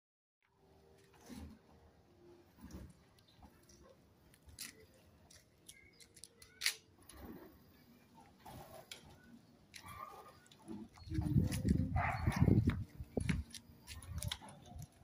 How to drive an Electric Bike | Mary Jane Aduna
10 year old driving an Electric Bike
EBike
How to drive an Electric Bike
Driving Skils
Alternative Vehicle